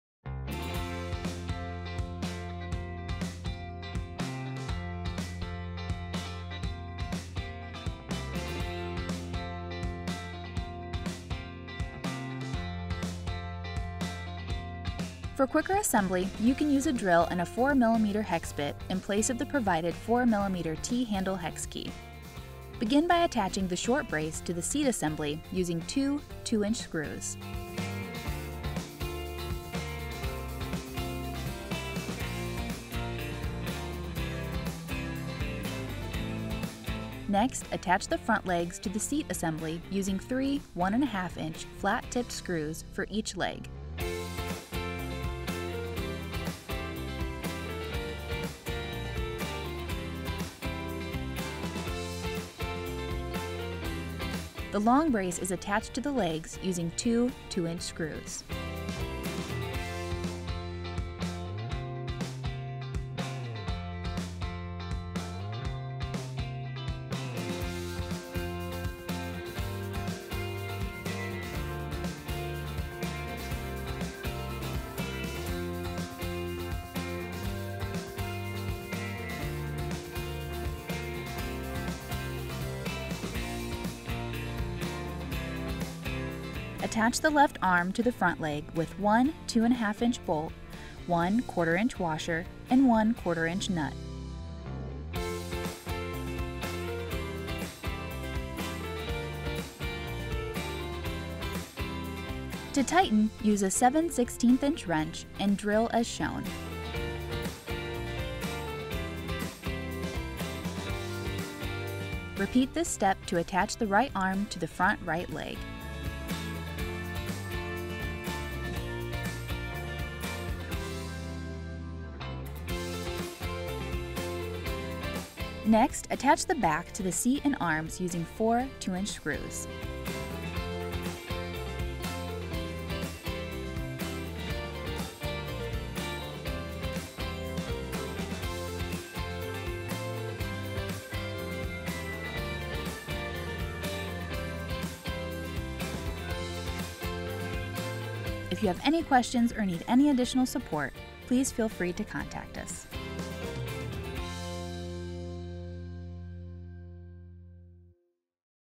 POLYWOOD® HNA15 - Palm Coast Ultimate Adirondack Assembly Video
Assembly instructions for the HNA15 Palm Coast Ultimate Adirondack.